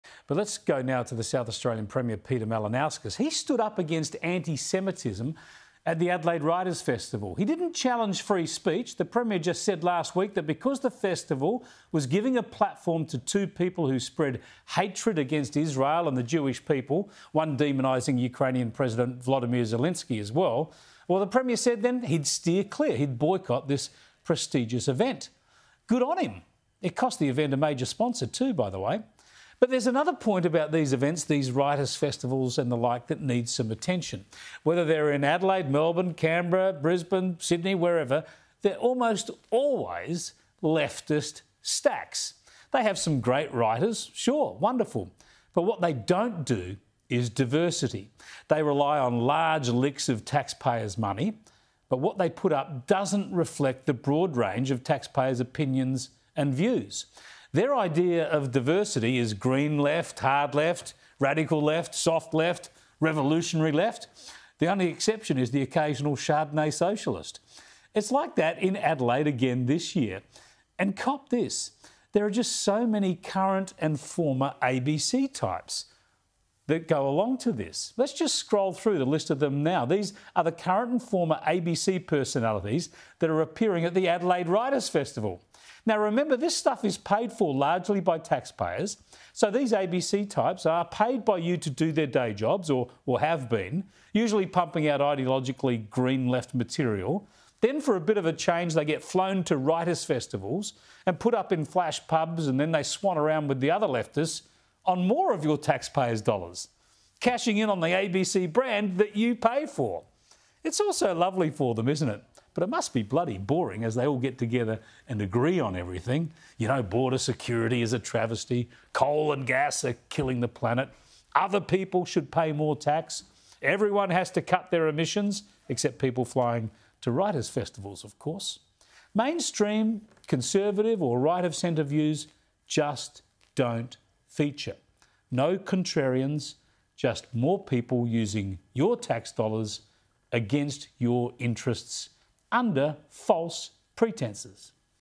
Malinauskas ‘stood up against anti-Semitism' at Writers Festival
Sky News host Chris Kenny says South Australian Premier Peter Malinauskas “stood up against anti-Semitism” at the Adelaide Writers Festival.

“He didn't challenge free speech,” Mr Kenny said.

“The Premier just said last week that because the festival was giving a platform to two people who spread hatred against Israel and the Jewish people, one demonising Ukrainian President Volodymyr Zelensky as well, well the Premier said then he'd steer clear, he’d boycott this prestigious event.

“Good on him. It cost the event a major sponsor too.”

Mr Kenny said events such as writers festivals rely on “large licks of taxpayers’ money” but what they put up doesn’t reflect the broad range of taxpayers’ opinions and views.